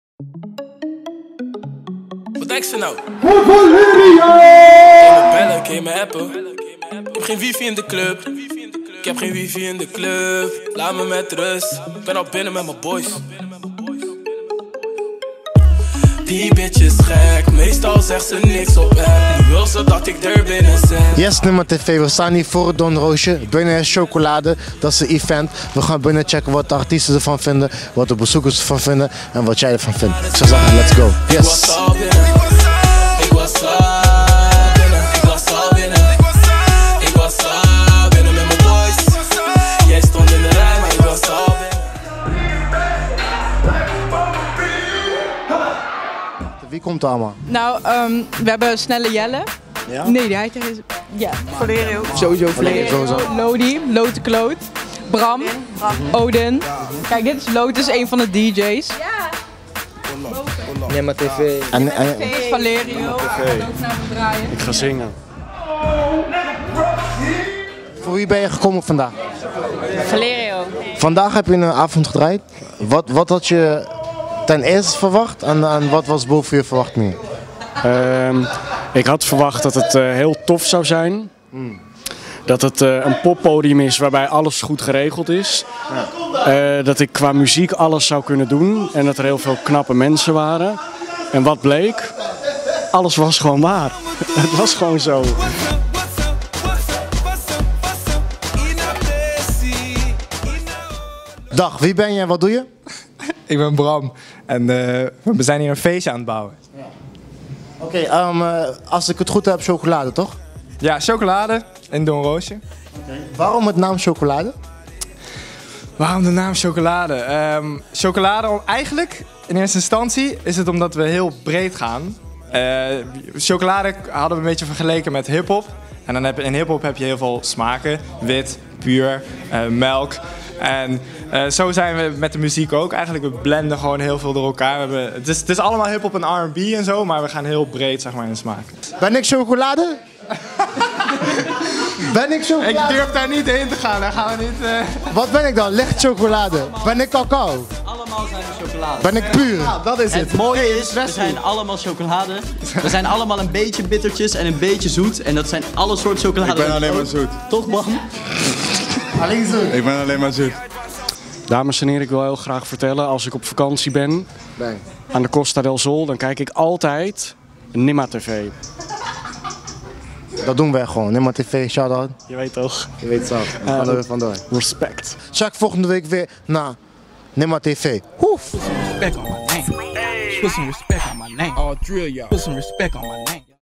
Chocolade - Doornroosje 2016
Wij van NimmaTv zijn bij het feest geweest in het DoornRoosje van Chocolade. kijk je mee?